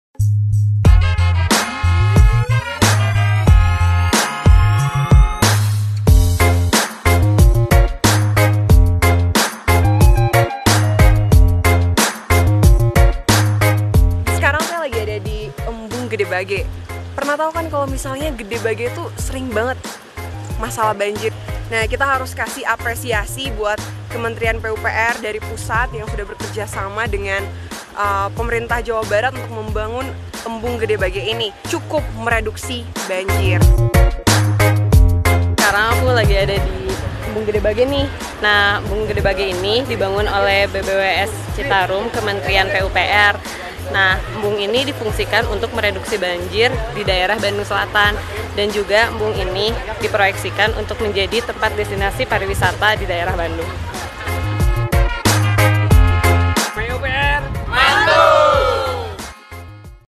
Embung Gedebage
Embung Gedebage, Bandung Timur, Jawa Barat. Embung ini sudah rampung pembangunannya.

Sebelum ada embung, kawasan Gedebagr dan sekitarnya sering banjir dan lama surutnya. Maka, Kementerian PUPR membangun Embung Gedebage tersebut.

Selain untuk meminimalkan banjir, Embung Gedebage menjadi tempat rekreasi warga setempat. Dari ini kegiatan ekonomi tumbuh. Ada jualan, mancing ikan, dan lain-lain.

Courtesy: Kementerian PUPR